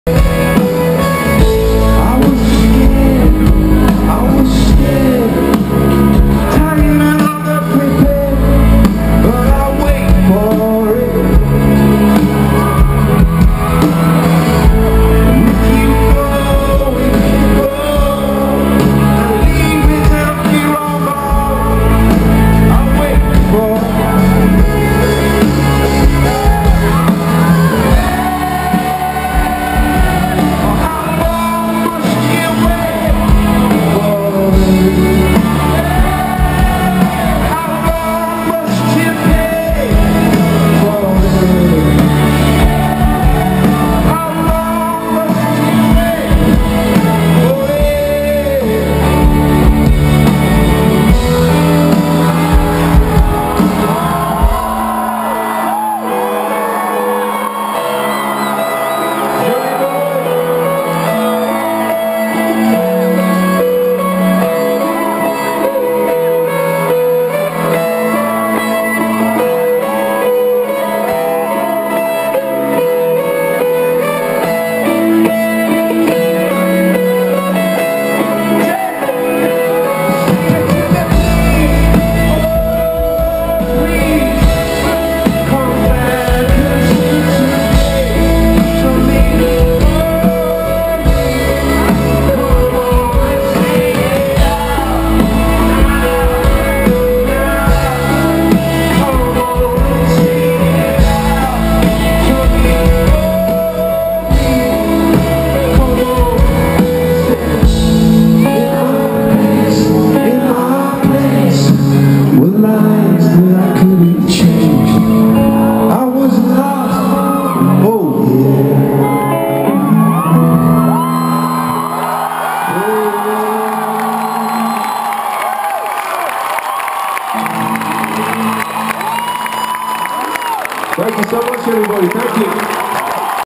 Coldplay - Zurich 26.05.2012
Coldplay concert in zurich letzigrund stadio go pro hero 2